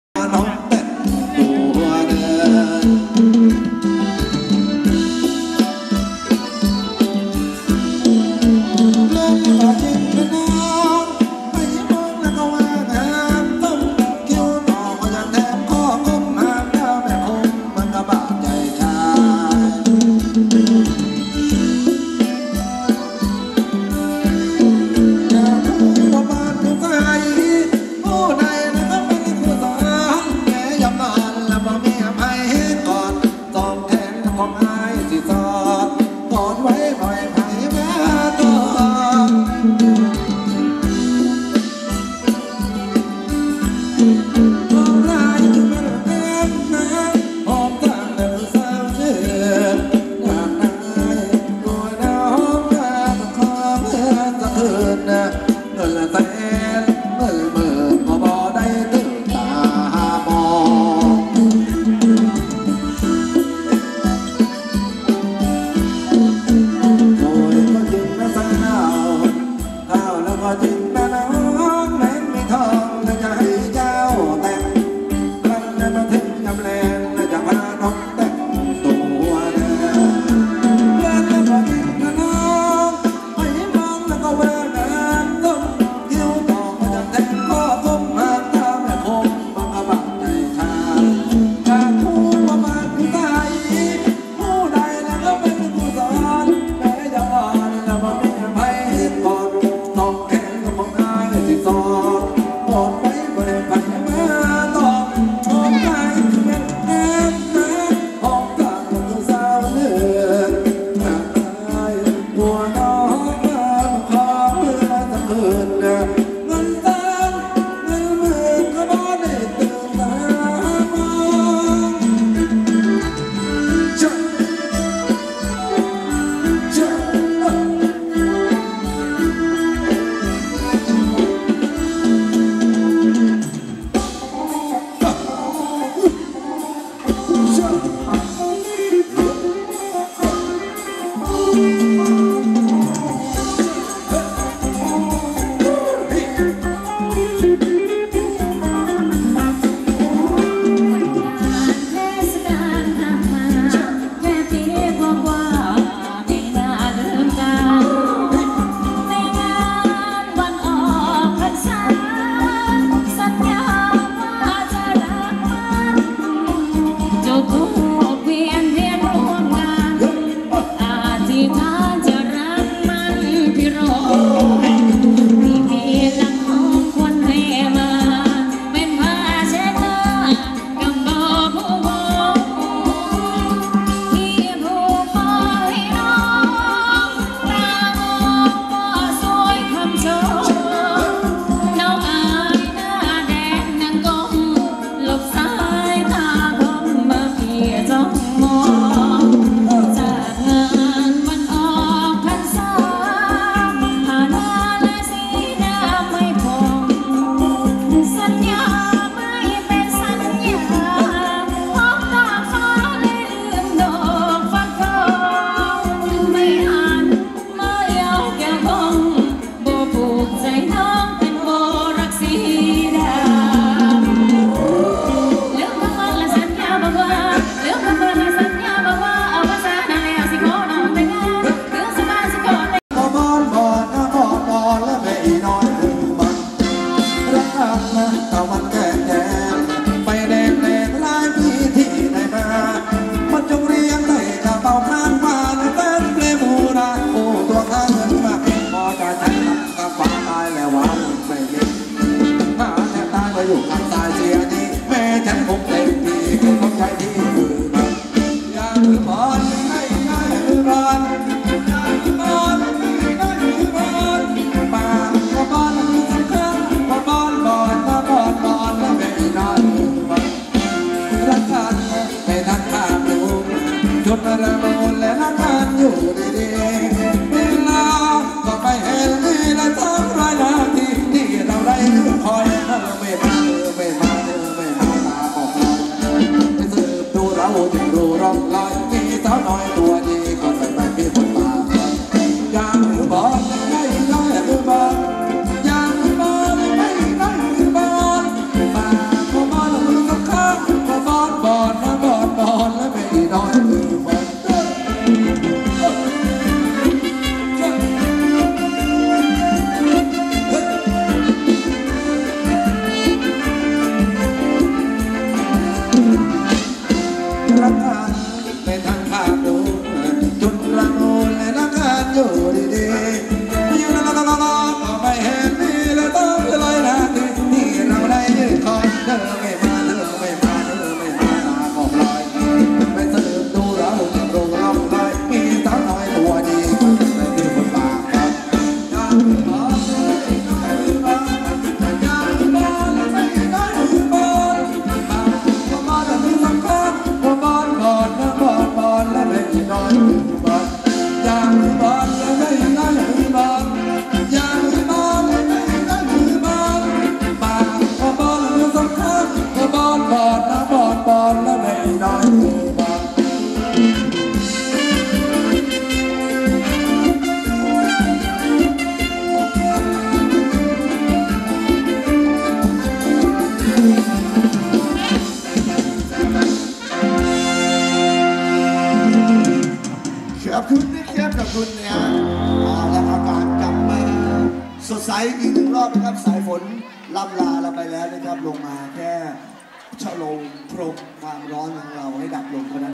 [Olympus]【PhetchaburiDance Workout for weight loss】รำวงเพชรบุรี✿ไมค์ AZDEN SGM-990+i✿อย่ามือบอน❀
name of Dancer 7 team : Nongbonapaporn,Namtalwhanmuangphet,Rungnapamuangphet,Phetsopha,Pornprommuangphet,Pornsawan,Pernphitwongjinda
name of Music Band : Jassmin
Show location : WatLadpho,Phetchaburi,Near Hua Hin,Thailand